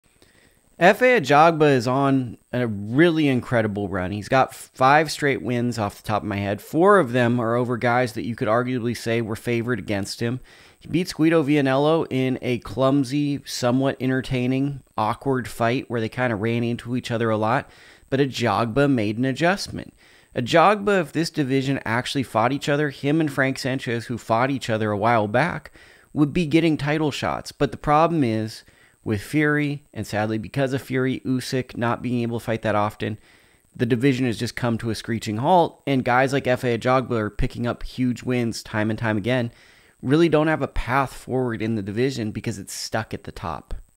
Efe Ajagba's Career Revival Continues, Beats Guido Vianello
Thoughts on this heavyweight bout